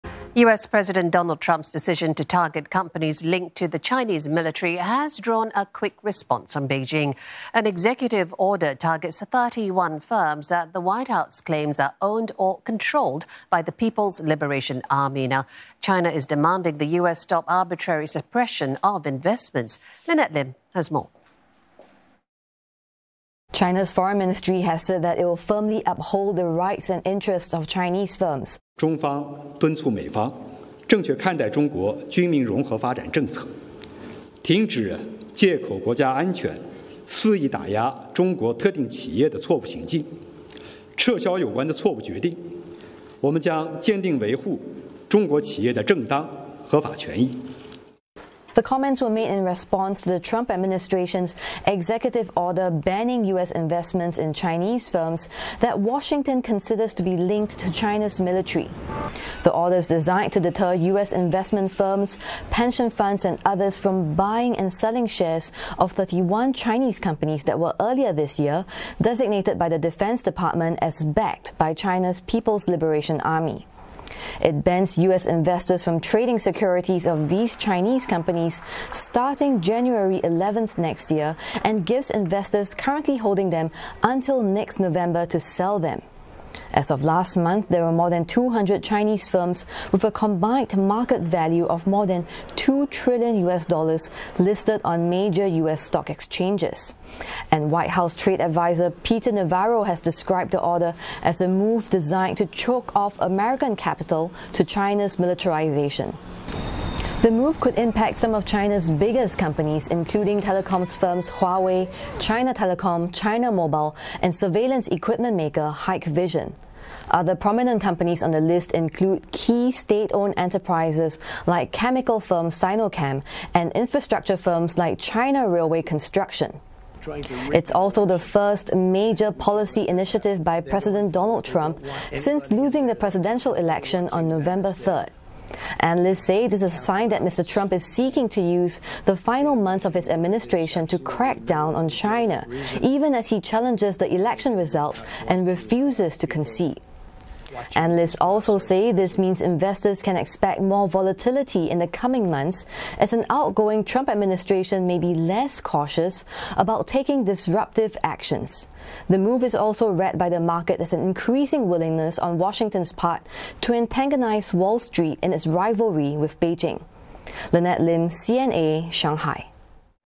China demands US stop arbitrary suppression of investments
US President Donald Trump's decision to target companies linked to the Chinese military has drawn a quick response from Beijing. An executive order targets 31 firms that the White House claims are owned, or controlled, by the People's Liberation Army. China is demanding the US stop arbitrary suppression of investments.